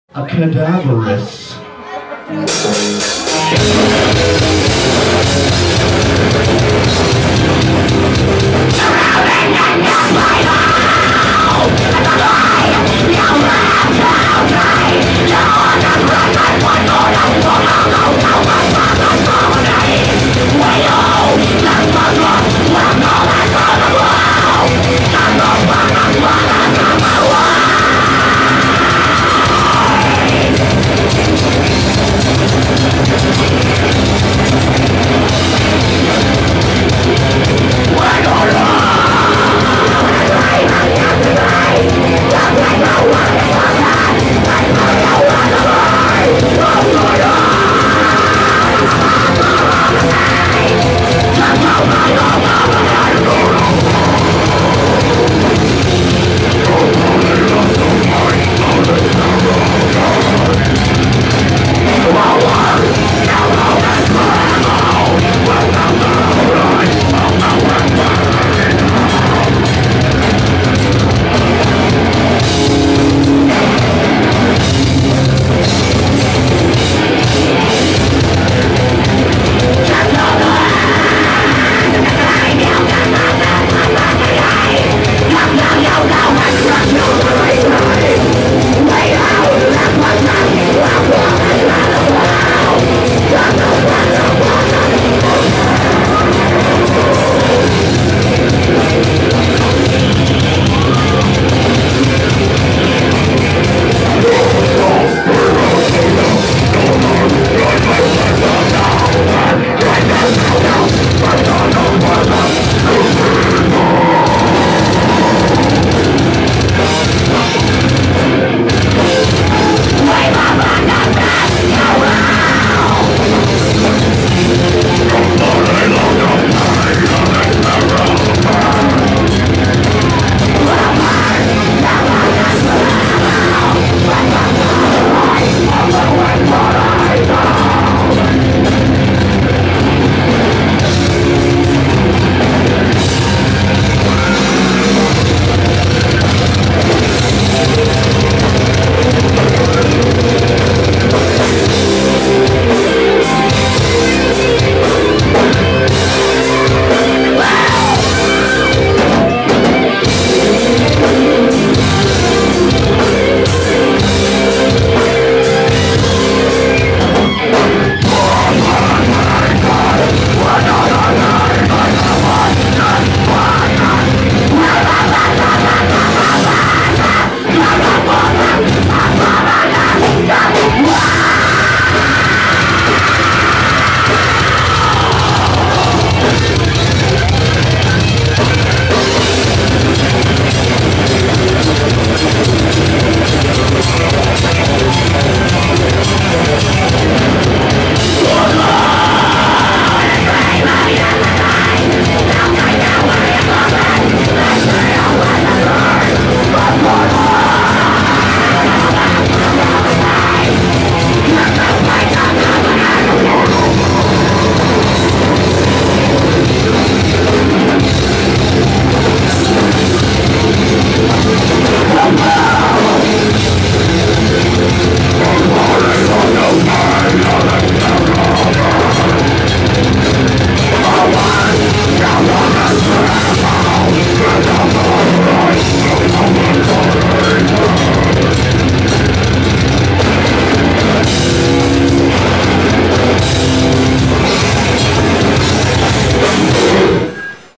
Mentacide-7/31/10
Mentacide,Burning at the Stake,Fatalist, and Rings of Saturn at Take 2 in Ventura California.